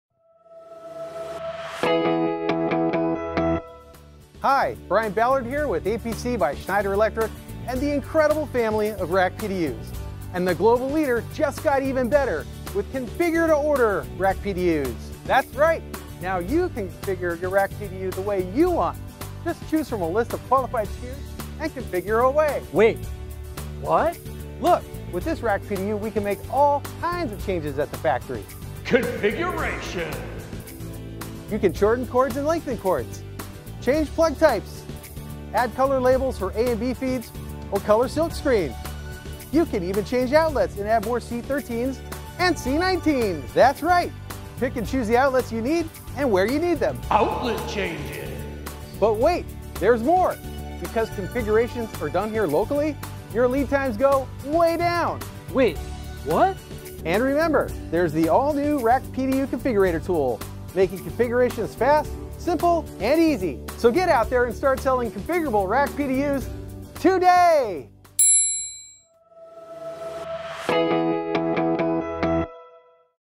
Configure to Order Rack PDU Overview and How to Configure a PDU | Schneider Electric
Discover the new Configure-to-Order Rack PDU and how it can revolutionize your data center power distribution! With APC's Configure-to-Order architecture, you can customize your Rack Power Distribution Units (PDU) to perfectly suit your needs. This video provides an overview of the configuration process using the Design Portal Rack PDU Configurator from APC by Schneider Electric. Learn how our in-region manufacturing ensures faster delivery and meets unique project requirements, offering thousands of configurable options, including outlet configurations, layouts, colors, and input cords of various lengths.

Whether you need a quick customization or a specific power distribution setup, our Configure-to-Order Rack PDU offers unparalleled flexibility and efficiency. Watch the video to see how easy it is to configure a Rack PDU and streamline your power management needs.

🔔 Timestamps:
0:01 - Introduction and Overview
0:22 - Factory Configuration Details
0:49 - Benefits and Conclusion

🔗Learn more about the Factory Configurable Rack PDU and other available customization capabilities

👋 GET TO KNOW US BETTER